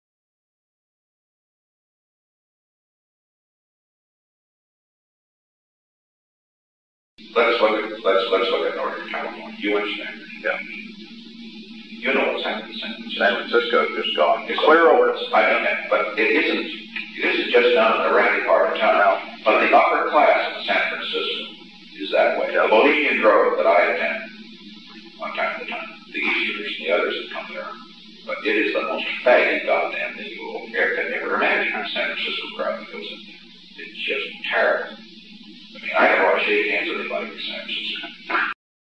Richard Nixon - About Bohemian Grove 5/13 -1971
"The Bohemian Grove, which I attend from time to time—it is the most faggy goddamned thing you could ever imagine."